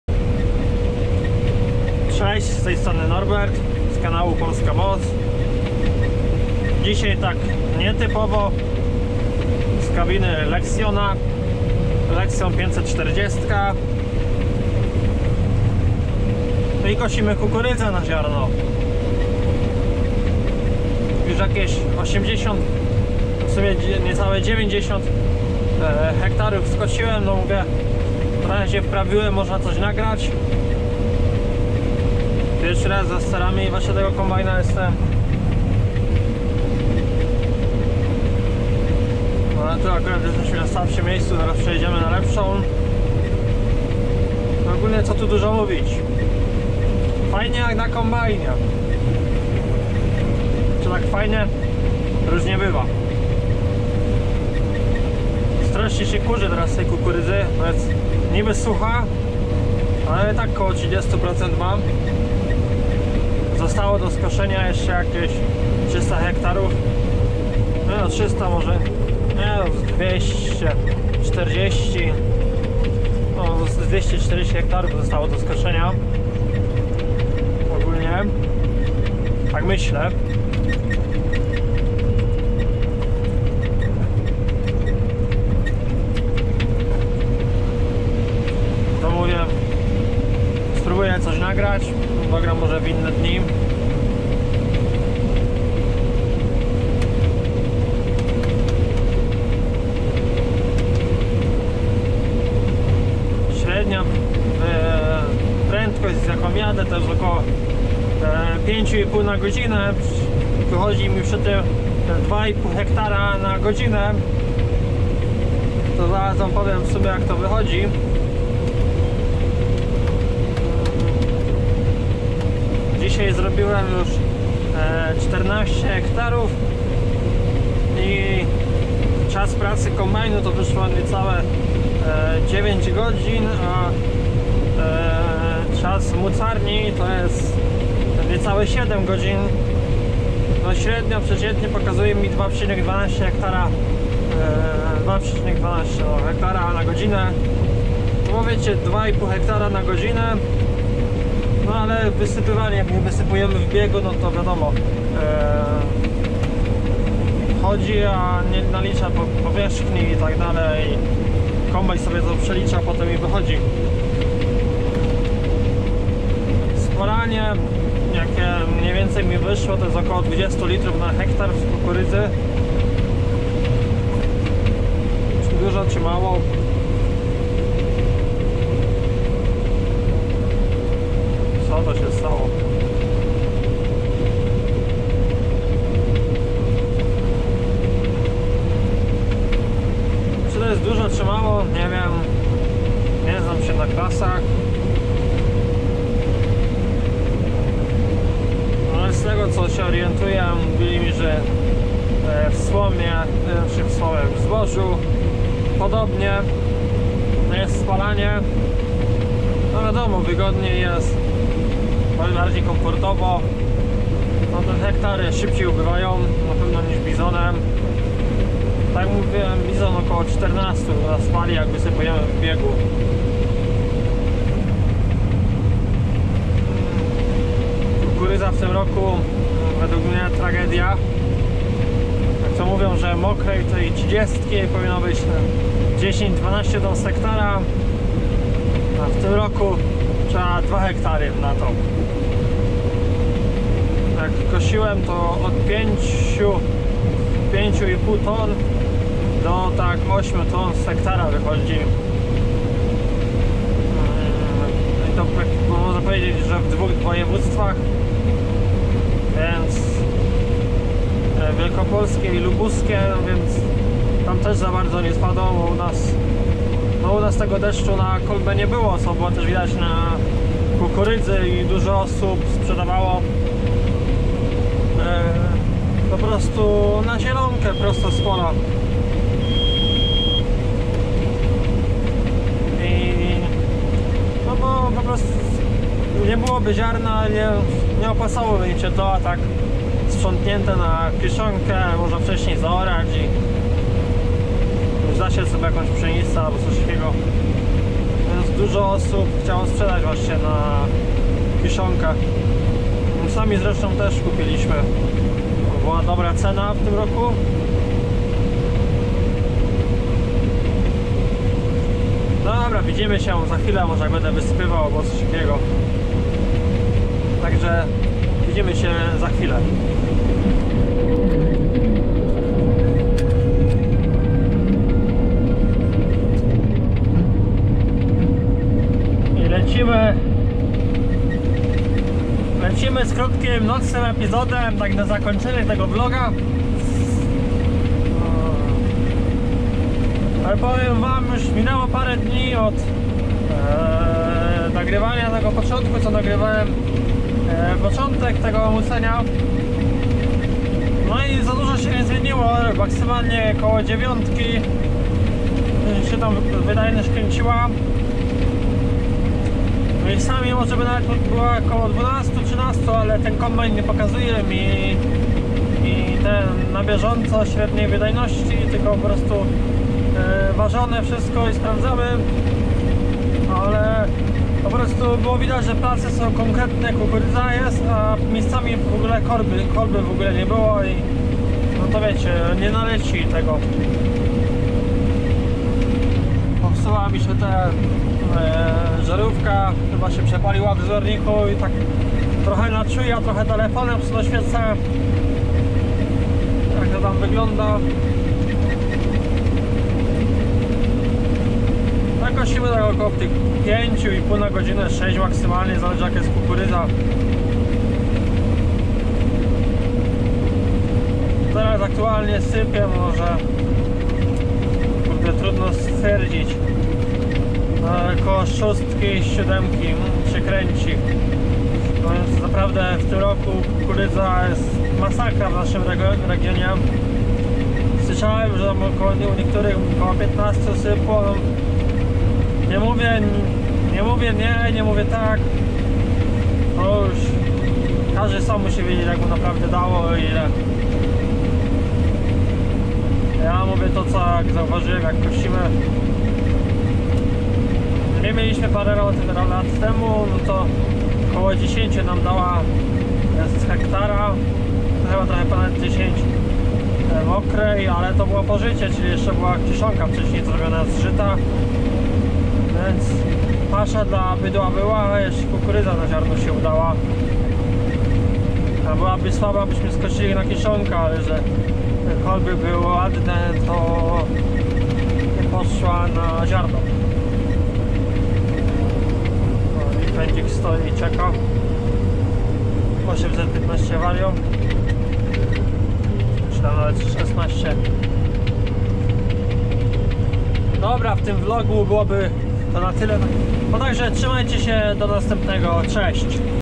☆Fajnie jak na kombajnie?!☆Żniwa kukurydziane2020☆Claas Lexion 540☆
Żniwa kykurydziane  dobiegły końca,  więc jest czas na film,a w nim dowiecie  się jakie plony spalanie,  i jak wygląda u nas sytuacja  :D